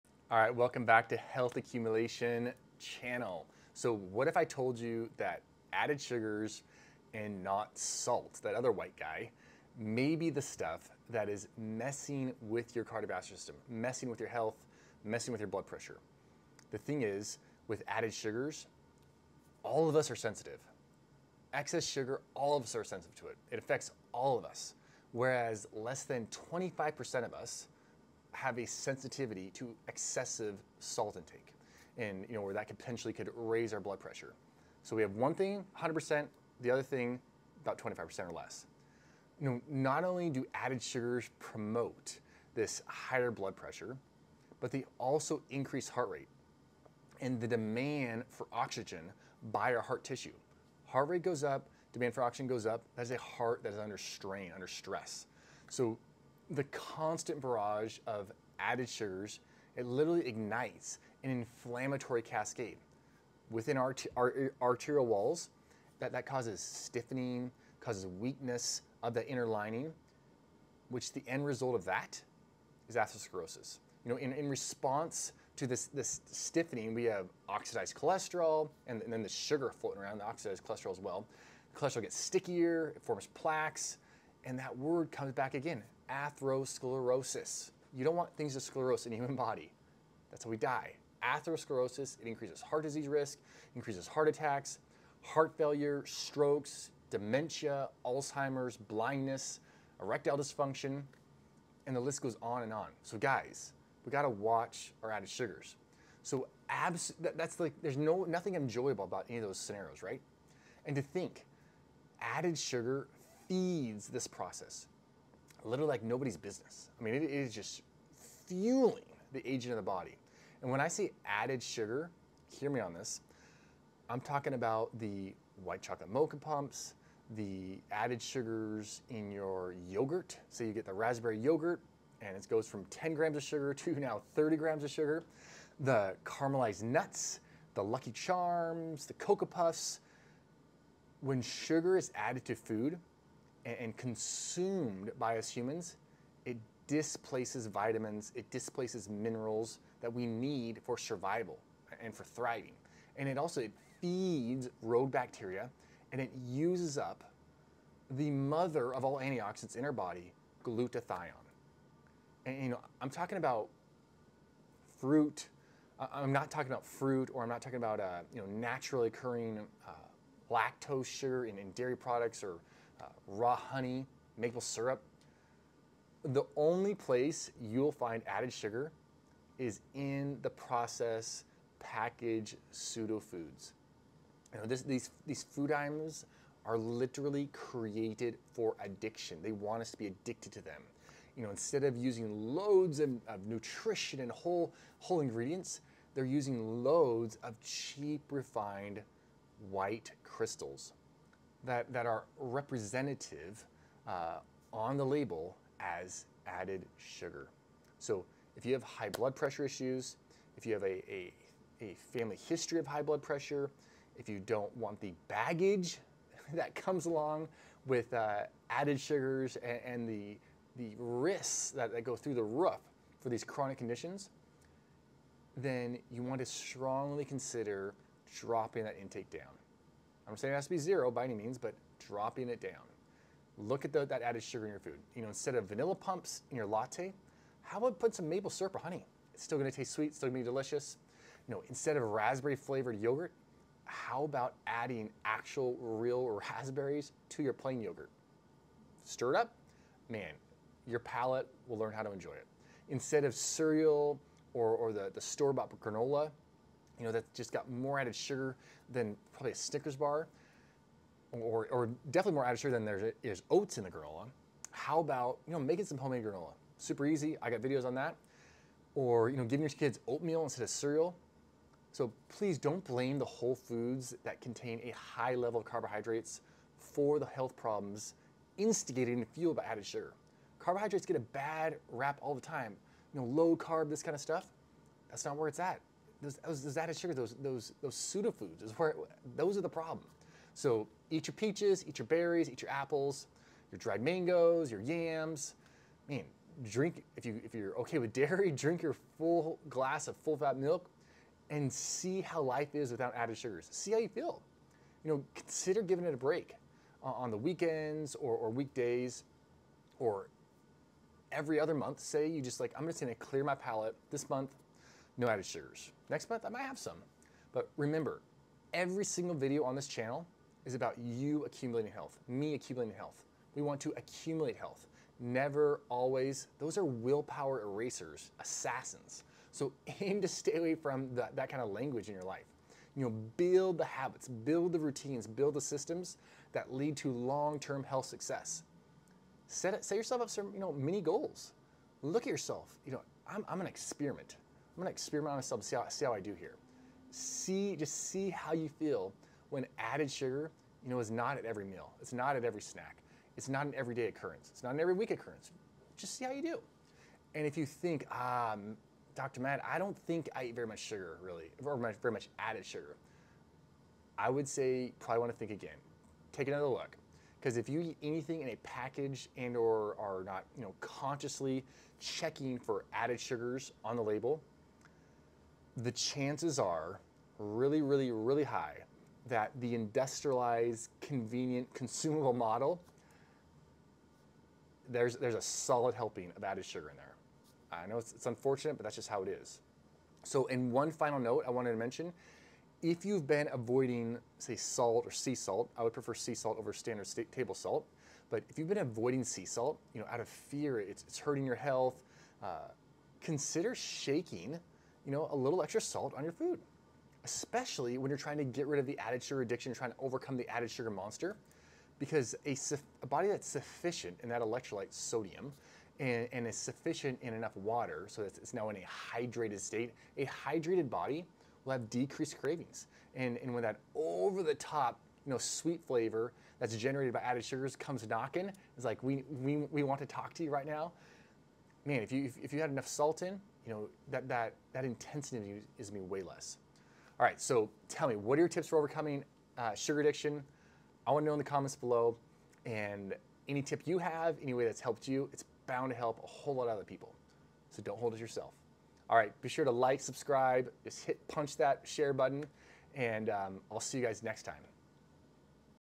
Does Sugar Increase Blood Pressure?
What if I told you that added sugars and not salt may be the white stuff that is messing with your cardiovascular system. Not only do added sugars promote higher blood pressure but they also increase heart rate and the demand for oxygen by the heart.

The constant barrage of added sugars ignites an inflammatory cascade within your arterial walls, causing stiffening and weakening of the inner lining.

If you have high blood pressure issues or if you have a family history of high blood pressure, you don’t want the baggage that comes along with added sugars.

Look at yourself as an experiment.  See how you feel when added sugar is not at every meal or not an everyday occurrence.

Download 12 easy and delicious recipes the whole family will love.